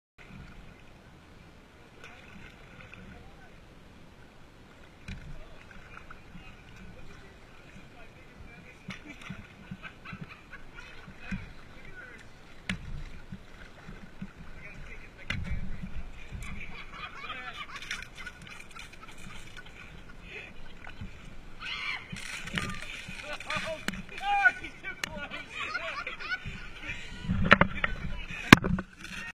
Supersoaker Fight July 4th 2017
Sea based bombing did well until an aerial assault.